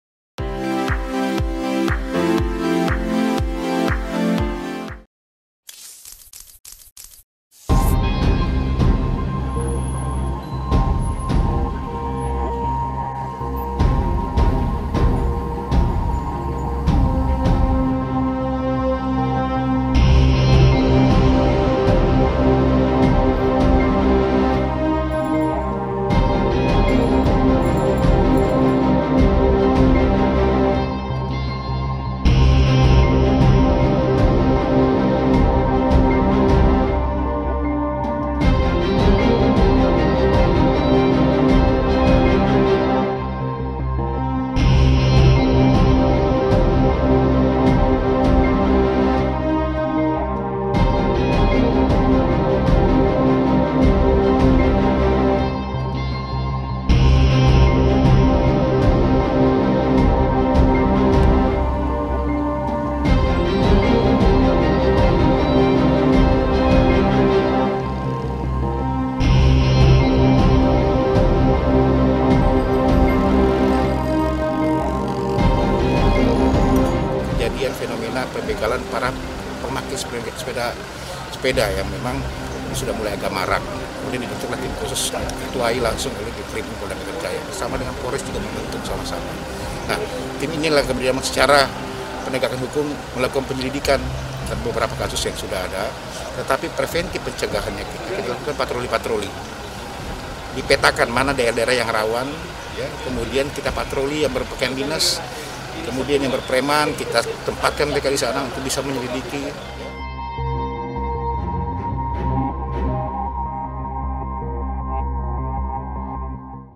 Rawan Begal, Warga Diimbau Tak Bersepeda Sendiri
Aksi pembegalan yang menyasar pengguna sepeda terus terjadi di Ibu Kota. Pada Oktober 2020 tercatat sudah ada lima laporan yang masuk ke Polda Metro Jaya.

Salah satu laporan datang dari seorang anggota marinir berpangkat kolonel, Pangestu Widiatmoko saat bersepeda di Jalan Medan Merdeka Barat, Gambir, Jakarta Pusat. Kasus pembegalan Kolonel Pangestu menjadi perhatian lantaran terjadi di wilayah Istana Negara.

Laporan juga datang dari seorang warga bernama Hendra Gunawan yang menjadi korban begal saat bersepeda di Jalan Puri Indah Kembangan, Jakarta Barat. Aksi pelaku yang hendak merampas handphone korban sempat terekam CCTV.

Polda Metro Jaya lantas membentuk tim khusus untuk mengatasi maraknya begal terhadap pesepeda. Warga pun diimbau agar tidak bersepeda sendiri, mencari jalur aman serta tidak memasang handphone maupun tas di sepeda.